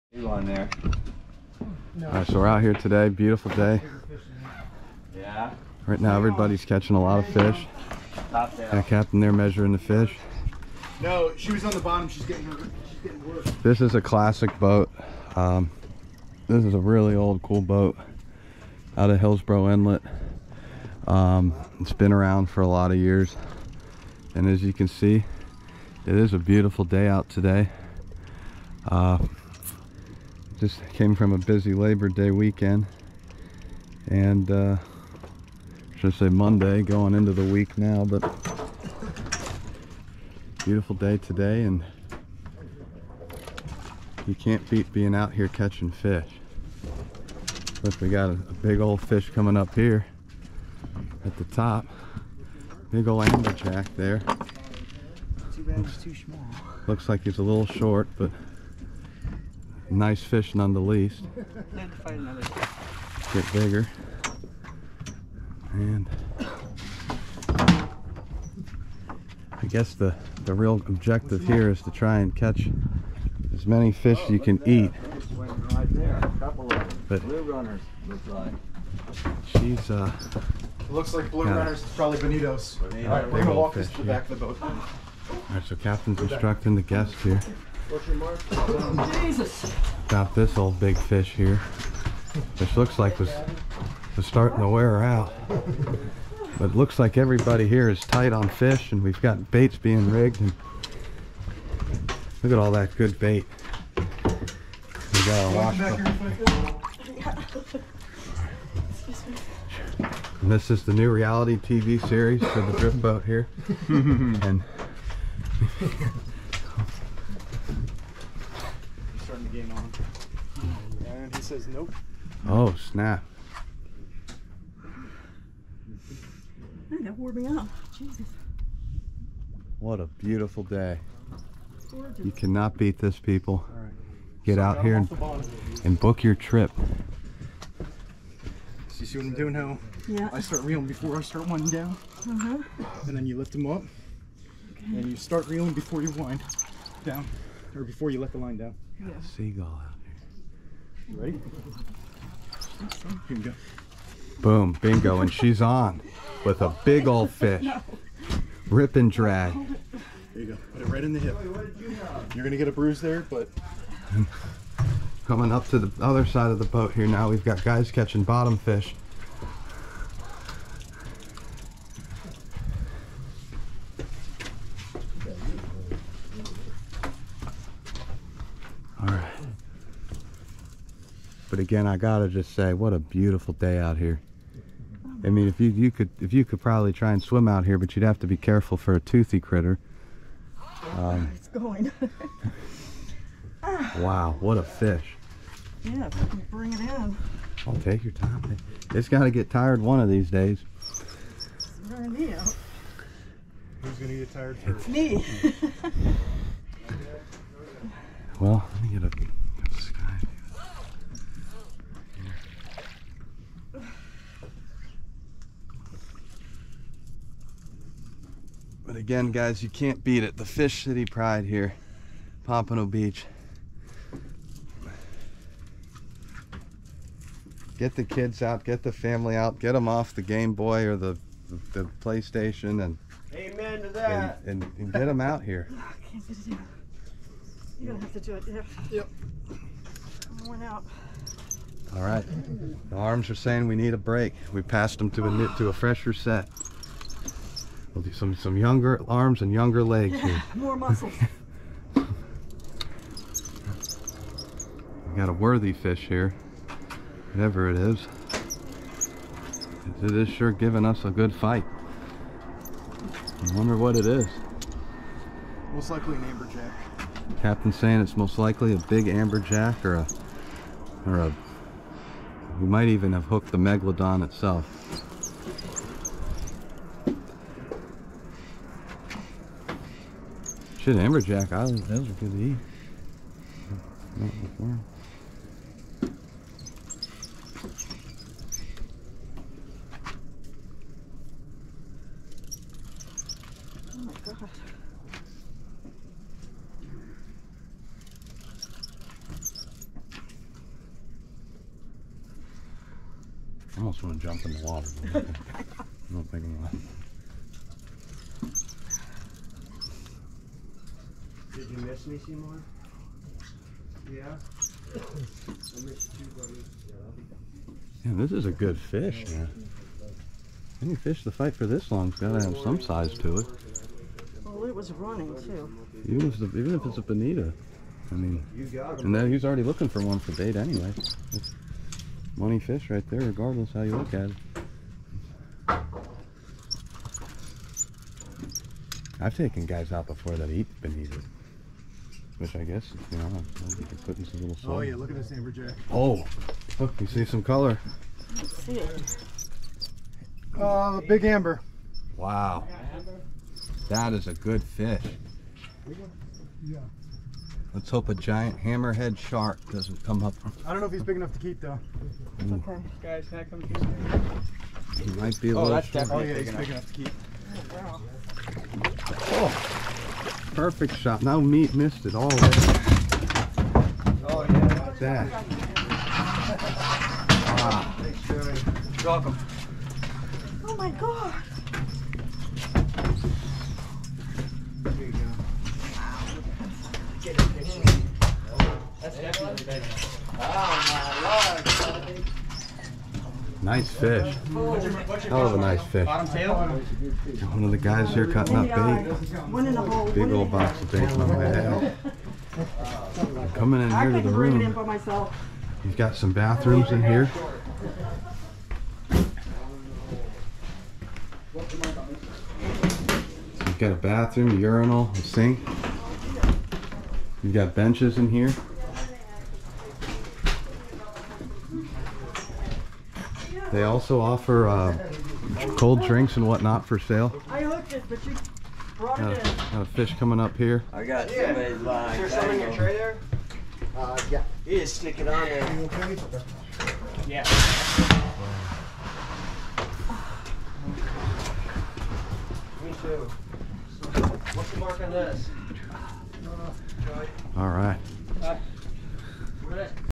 Fish City Pride Season 1 Episode 1 No Heimback Returns Vermillion Snapper and Amberjack
Hillsboro Inlet Drift Boat Reality Tv Show FISH CITY PRIDE only on YOUTUBE.COM. Produced by Shaughnessy Haas Enterprises LLC. You won't be disappointed.